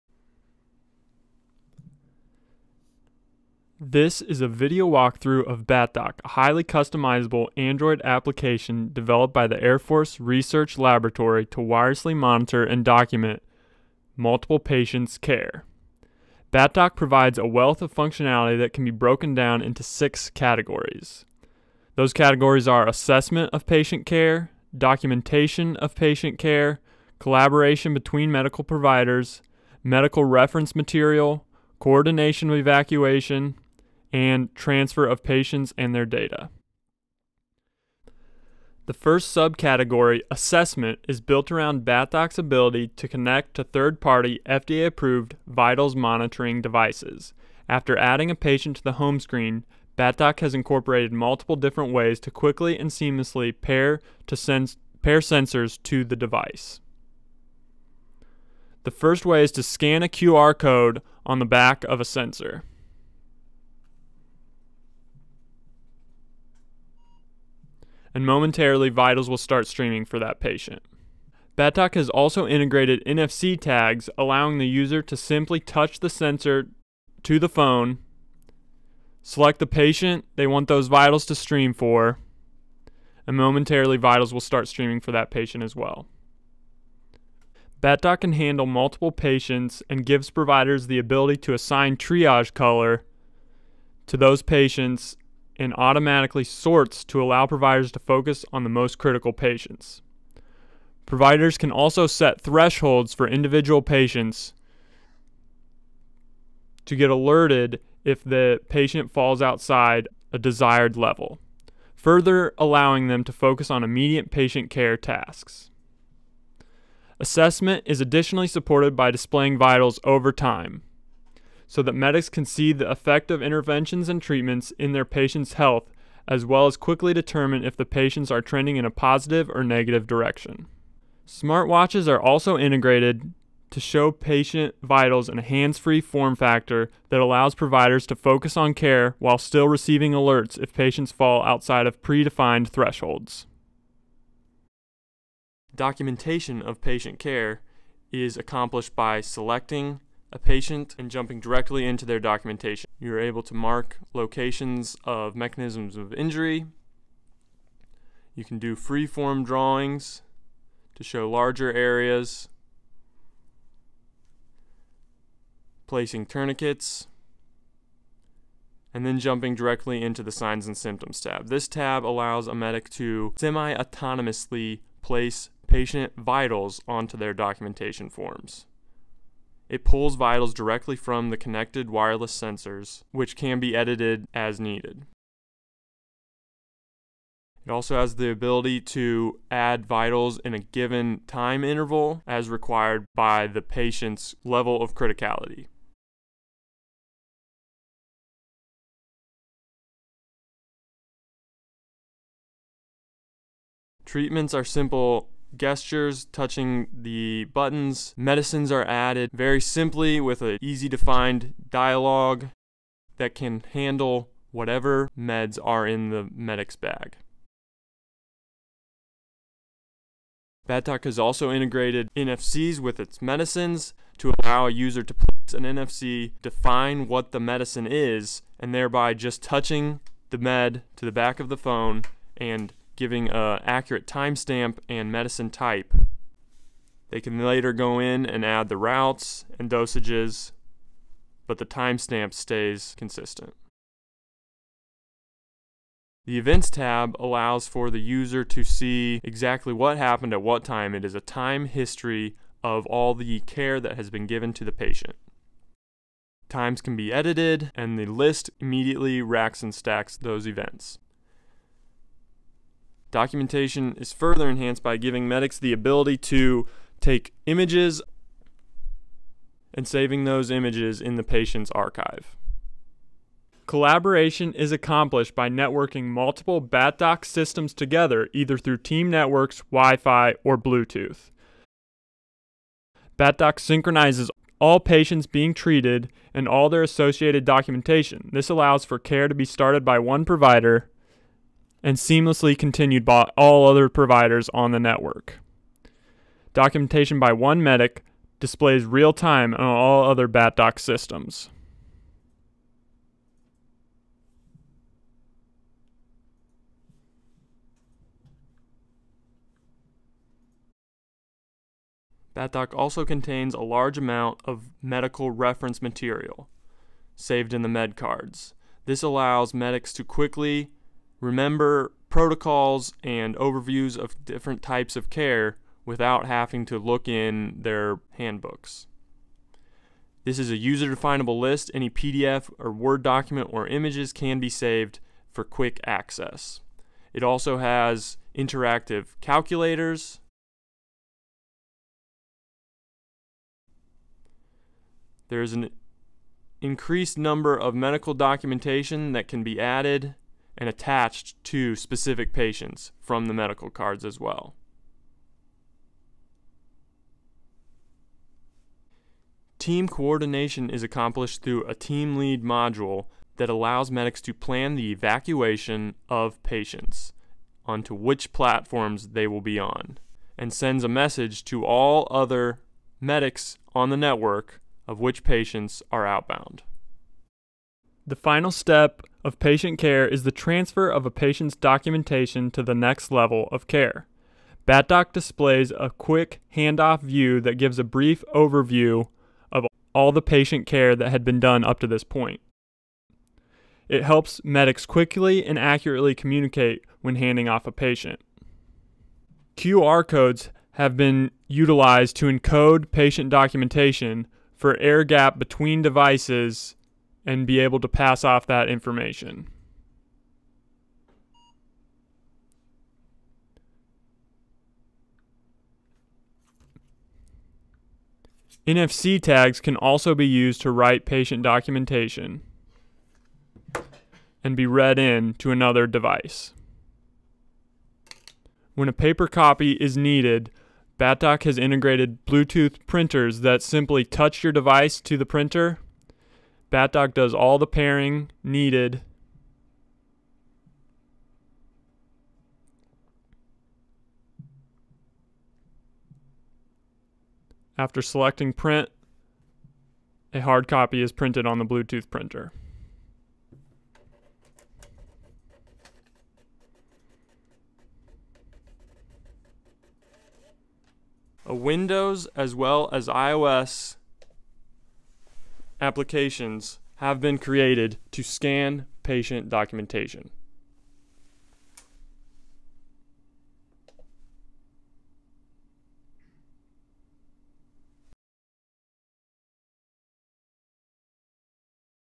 Technology: Battlefield Assisted Trauma Distribution Kit (BATDOK) Software
Invented by researchers in the Air Force Research Laboratory, Airman Systems Directorate, BATDOK software is a point-of-injury software tool. It enables medics to wirelessly monitor multiple patients’ vitals simultaneously at the point-of-injury, to capture a complete history of patient medical treatment from point-of-injury through medical evacuation, and follow-on transfer to the next level of care.

Refined with many iterations of medic use feedback, BATDOK leverages operator-centric, intuitive, easy-to-use mobile interfaces to facilitate maximum awareness and documentation of in-field patient care. BATDOKTM hosts a variety of multimodal covert/overt notifications, alerting the medic when patients cross predefined, user-set health thresholds. BATDOK can reference offline, current standard medical care procedures/guidelines through a drop and drag file structure accessed with single-click actions. Moreover, BATDOK was designed as an open architecture system to easily incorporate existing/emerging FDA wireless sensors, allowing the medic to select desired sensors for their unique mission. BATDOK is sensor and wireless protocol agnostic, allowing it to scale as new sensors and communications protocols come online.